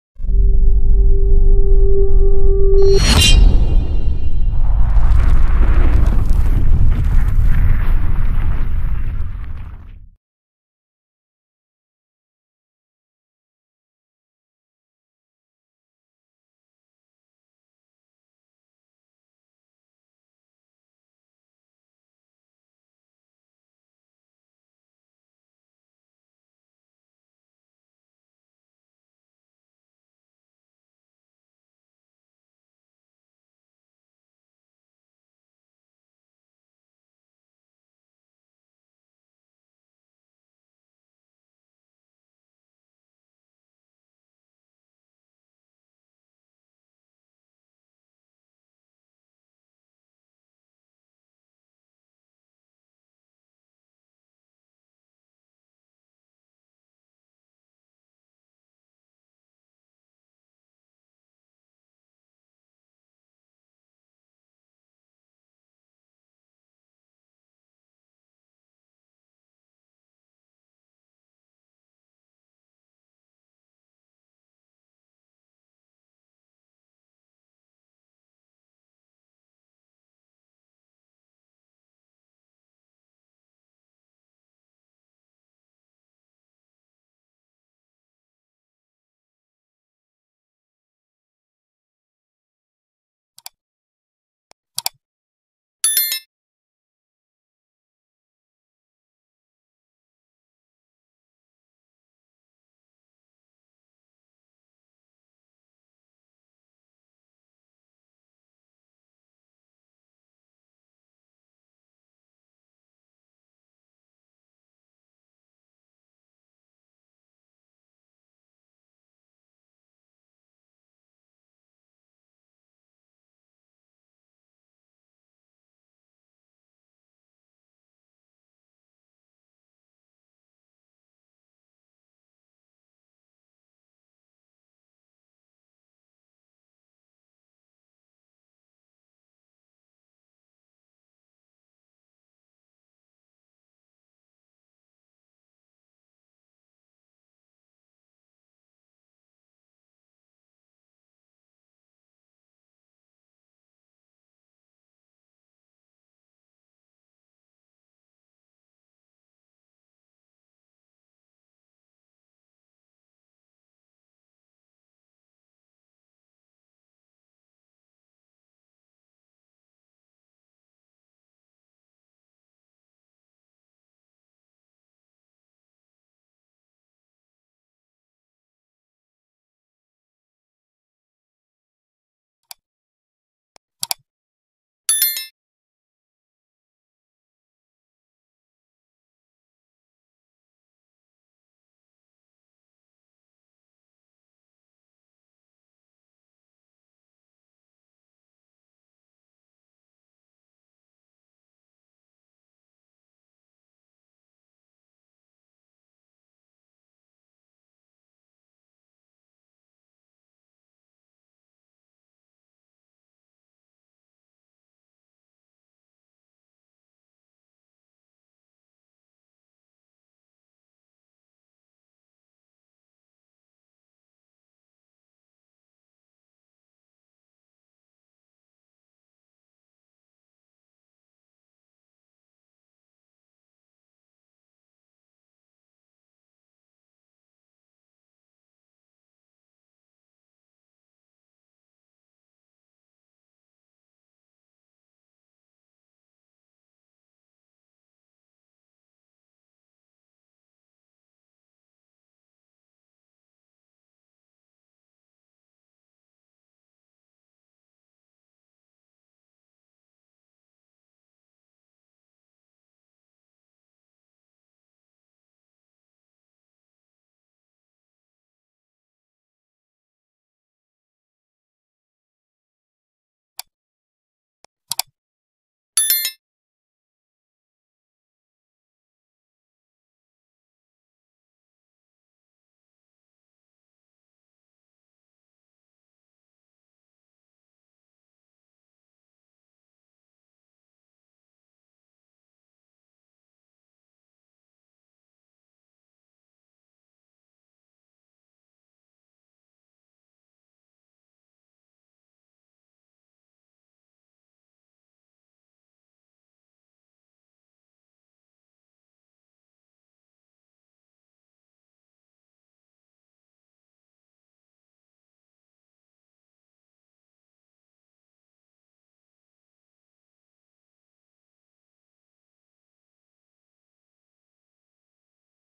HOW TO MAKE A PALINDROME NUMBER IN BLUE J|#4|SUPERB VIDEO|MUST WATCH
To learn how to make a palindrome checker in blue j watch the whole  video